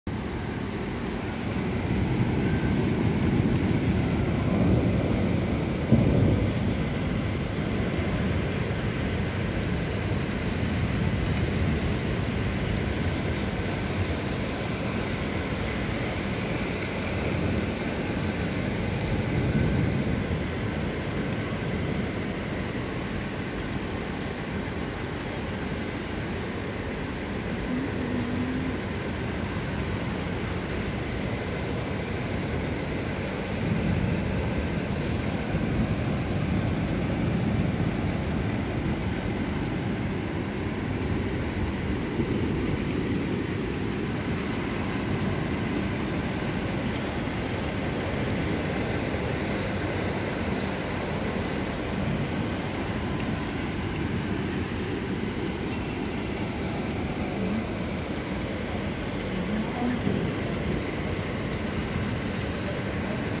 Happening now, heavy rain and first rain of November 2023
Happening now
02 November 2023
Heavy rain in the Kingdom of Bahrain

First rain of November 2023